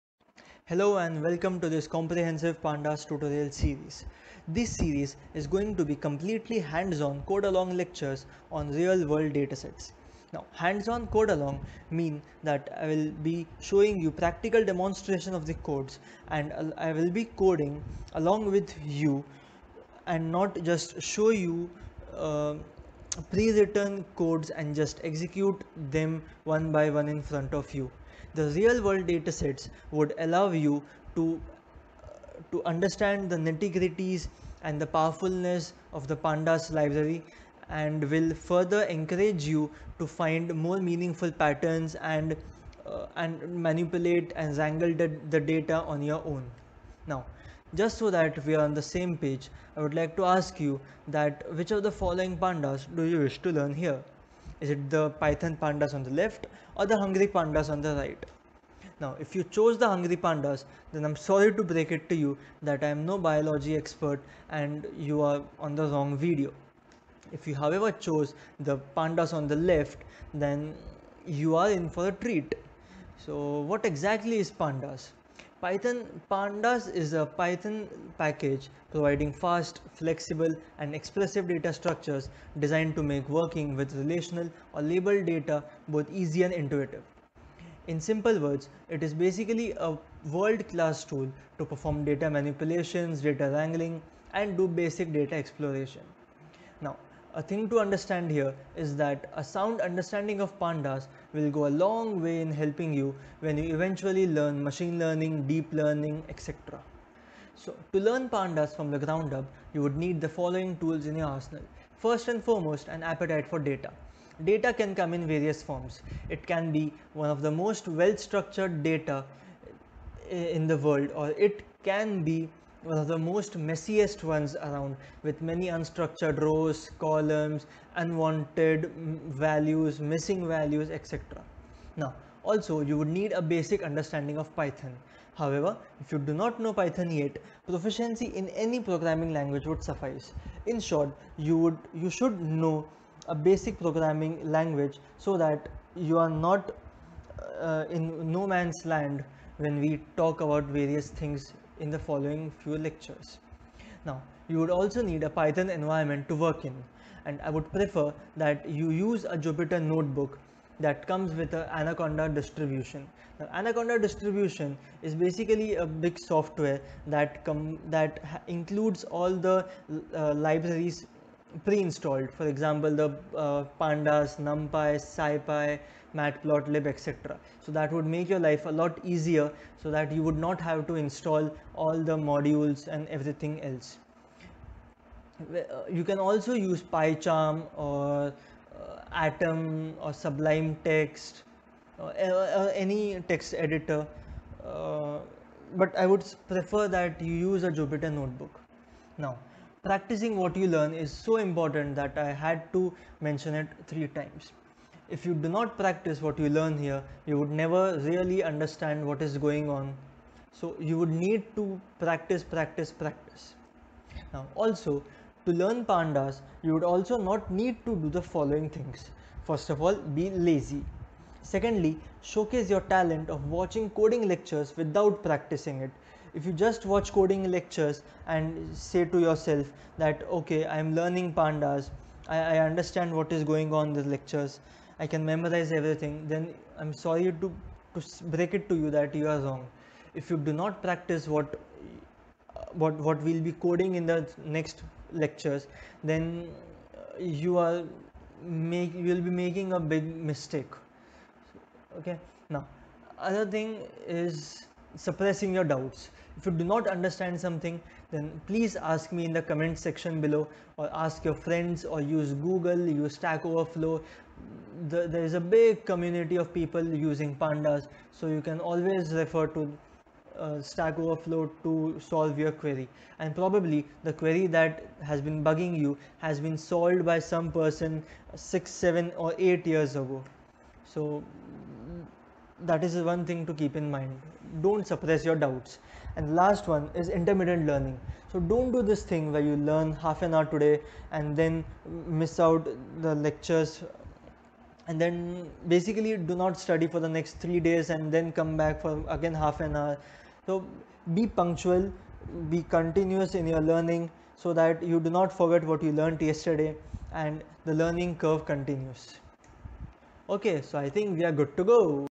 1 - pandas introduction
Welcome to this Comprehensive Pandas Tutorial series!

Before making a delicious dish, you would go through buying, washing, selecting, cutting, chopping and grinding vegetables before you can mix all of them up in perfect quantities and serve them fresh.

Pandas is your buying, washing, selecting, cutting, chopping and grinding step, albeit for datasets and not vegetables!

In this series, I'll walk you through all the necessary things you need to have in your arsenal to master this very important open-sourced data analytics and manipulation tool, which will eventually lay the foundations for an awesome Machine Learning and Deep Learning experience.

You can check out my github page to find all the relevant Jupyter Notebooks, datasets and other materials I'd upload with each video tutorial!